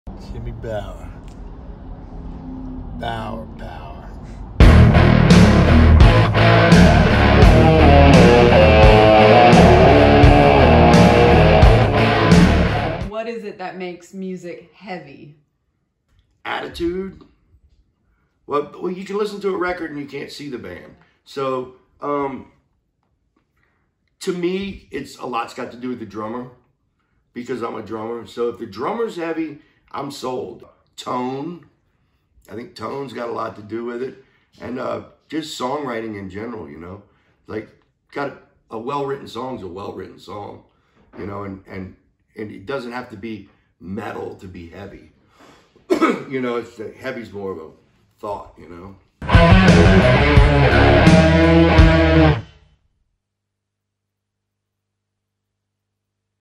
On the road filming the Wino Doc: Bower Power
Behind the scenes filming of the Scott Wino Weinrich Documentary, in production now.

Ask the Masters: with the legendary Jimmy Bower of Eyehategod, Down, and Mystic Crew of Clearlight.

Production: Sharlee Patches